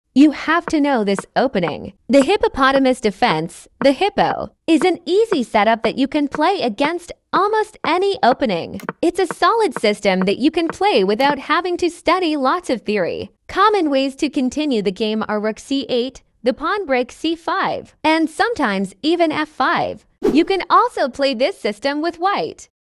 No opening theory needed with the HIPPO
The Hippo (short for Hippopotamus Defense) is a setup that can be used against almost any opening. You can get away with very little opening theory, and you can probably take your opponents out of theory rather quickly as well.

It's not the best opening if you're looking for perfect play, but it's definitely a solid one. You see masters (GMs, IMs, FMs, ...) play it all the time in blitz and bullet. Because of the simplicity of the setup, you can blitz out (or even premove, at your own risk) the first moves of the opening really fast and develop an early time advantage that could end up being crucial later on in the game.

It's even possible to play this system as White, the Hippopotamus Attack, but it's less common as it seems rather unambitious with the starting pieces. Nevertheless it gets you a similar position as you would get with Black with one extra tempo. So if your goal is to avoid theory, this might be a great option for you!